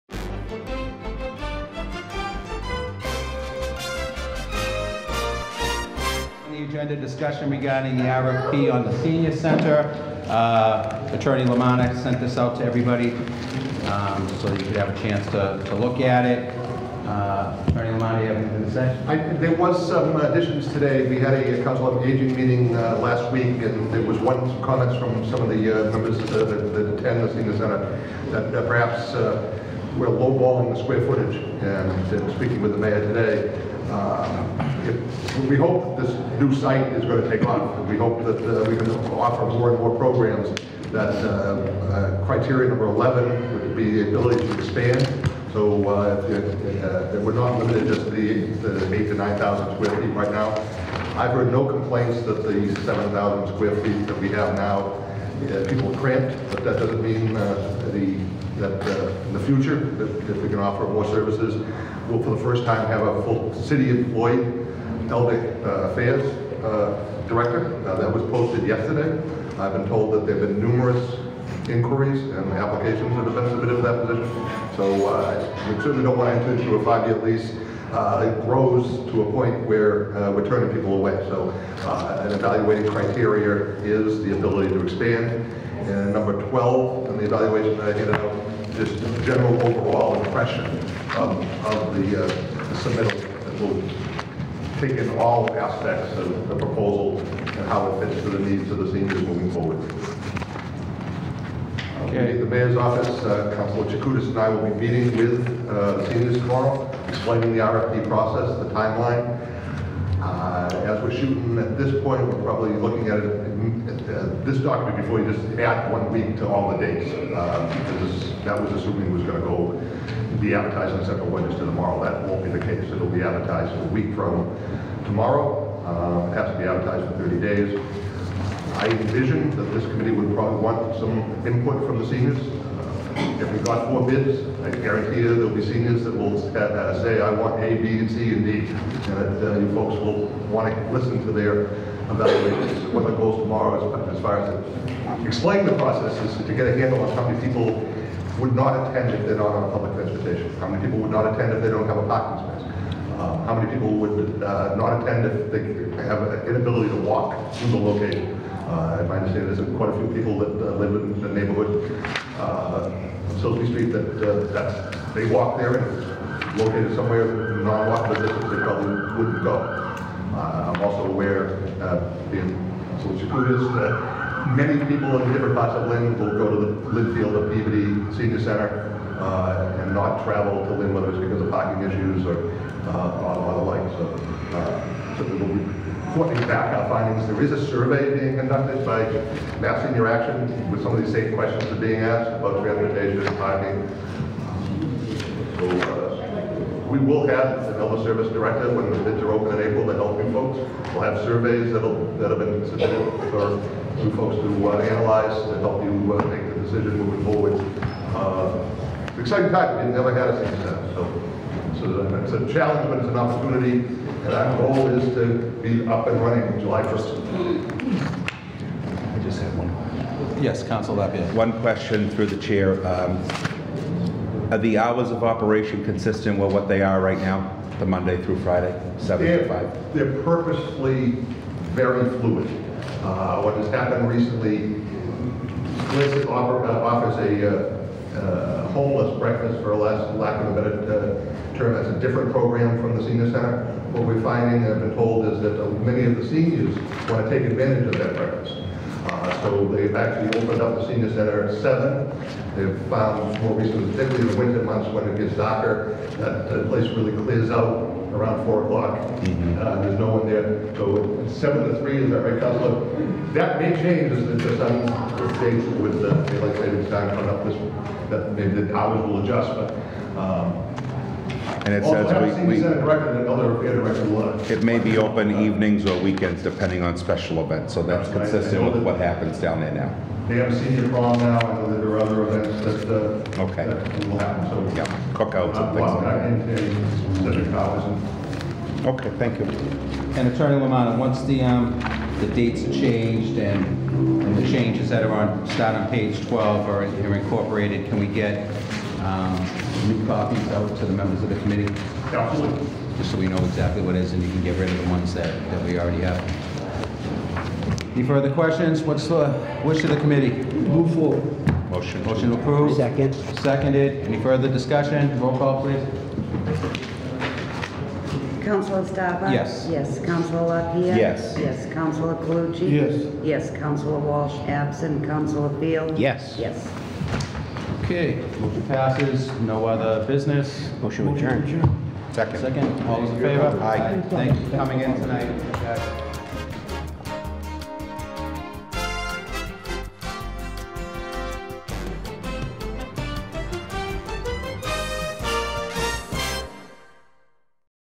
Lynn City Council Subcommittee Meetings | March 3, 2020
The various Lynn City Council Subcommittee Meetings from March 3, 2020. These meetings take place before the Lynn City Council Meetings. For the full schedule for the Council Subcommittees, and their agendas, you can

Find out more about Lynn Community Television:

Facebook.com/lynncommunitytv

Twitter.com/lynncommunitytv
YouTube.com/LynnTVO
rgInstagram.com/lynntelevision